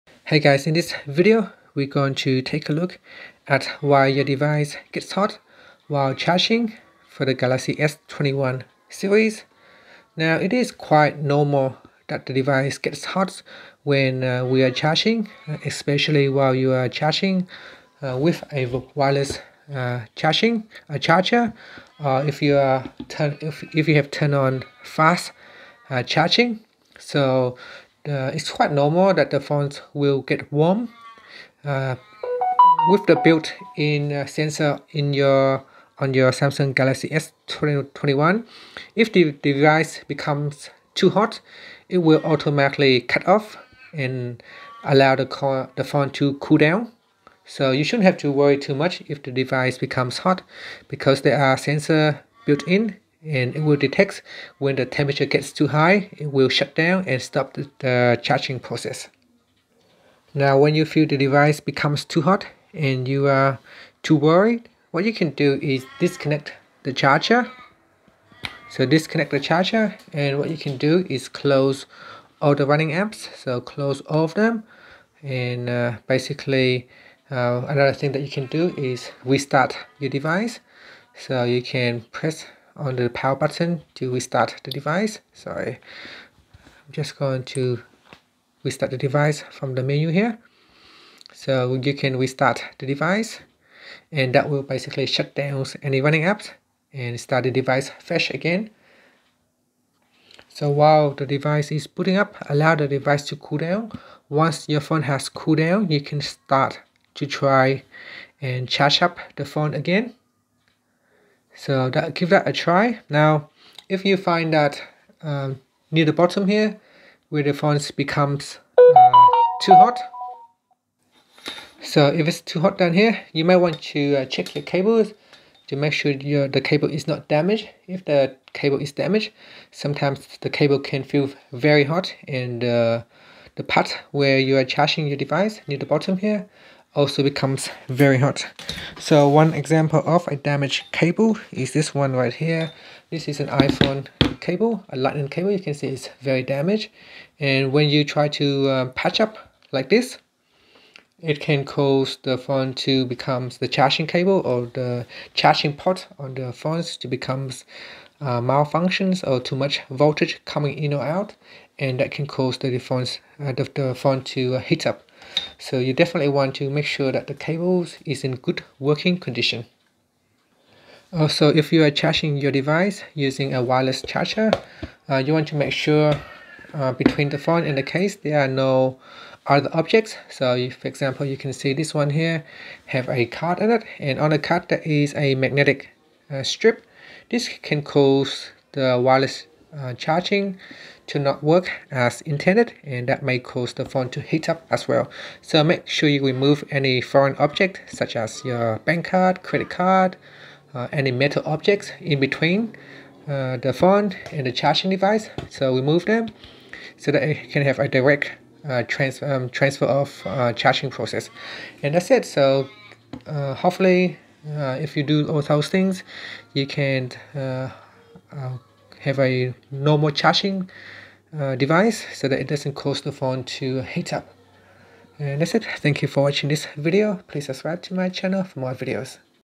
Why Your Samsung Phone Gets Hot While Charging and What Can You Do About It?
Learn why your Samsung phone gets hot while charging and what you can do about it. This can apply to your Samsung Galaxy S21, Galaxy S20, Galaxy Note 20, Note 10, Galaxy S10, S9, S8 and many others too.